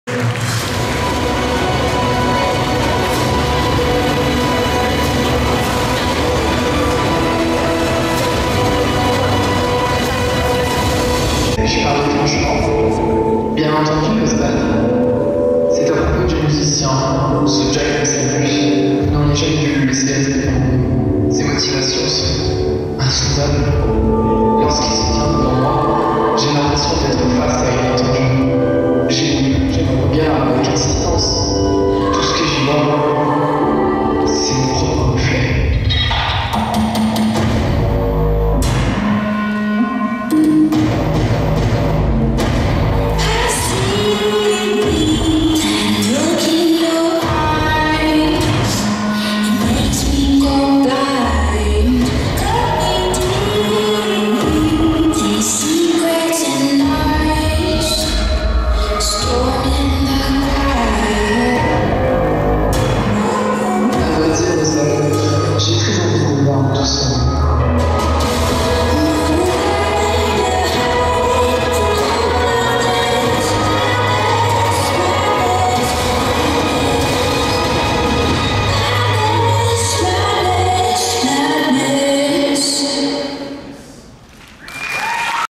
ANIMEST 2018 - 24/11/2018 - CONCOURS COSPLAY - 18S
Organisé par Bulle Japon

Présenté par Fairy

Jury's : Paku Paku Ru, DarkFF, Wanda Furiosa, Kana

Cosplayer : DESCH
Personnage : Jack Vessalius
Univers : Pandora Heart

Prix :